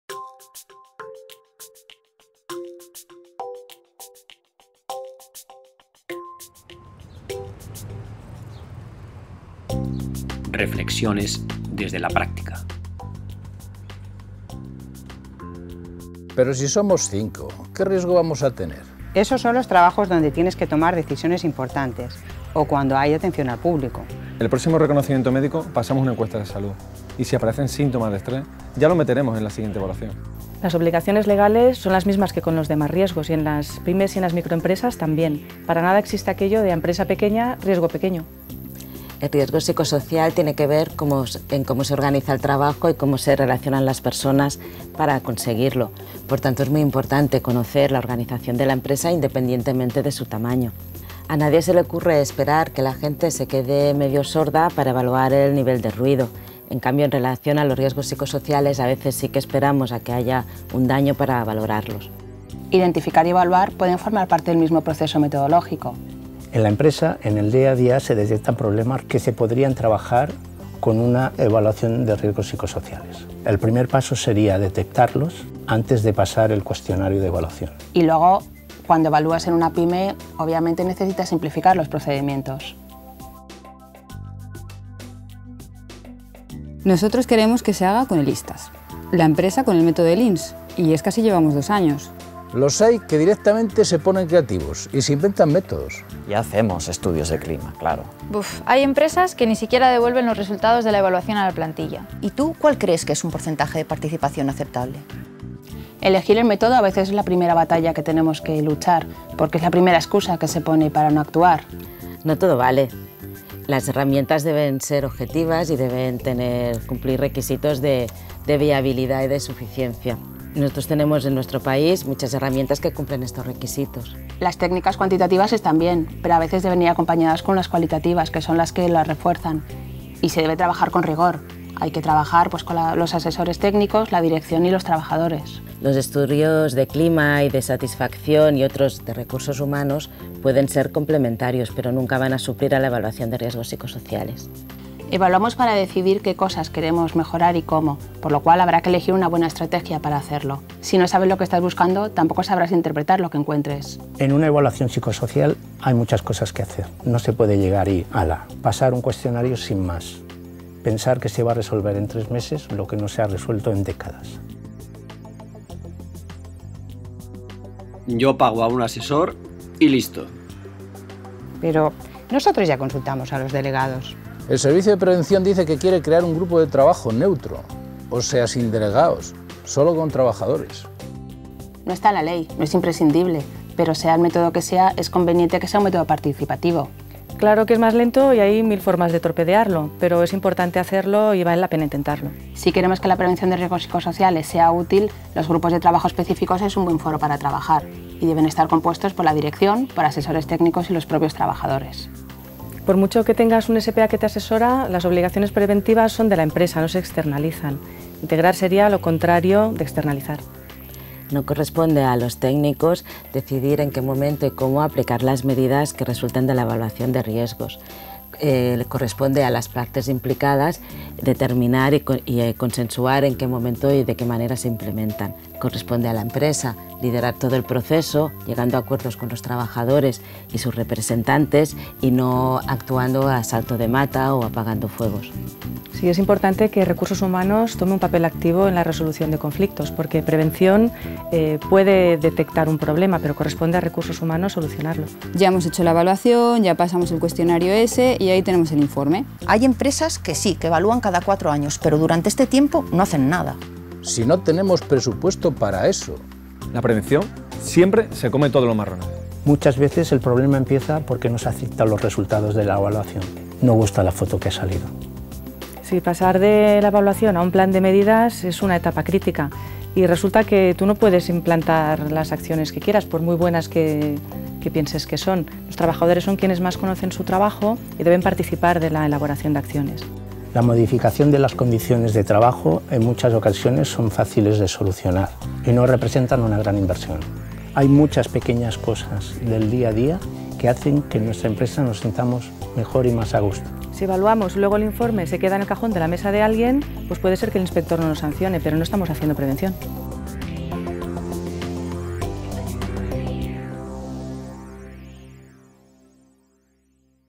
V. Reflexiones desde la práctica “Gestión riesgo psicosocial”
Se ha dado la voz a cuatro agentes con papel activo en el escenario de la PRL en nuestro país: empresa, inspección de trabajo, servicio de prevención ajeno y delegado de prevención. La conversación gira entorno a algunas de las dificultades/obstáculos habituales a la hora de gestionar la prevención de los riesgos psicosociales en la práctica.

V. Insights from practice. Psychosocial risk management

In this video four key OSH agents with an active role on OSH in our country (company, labor inspection, technical consultants and OSH representatives) discuss about some of the usual difficulties when managing the prevention of psychosocial risks in practice.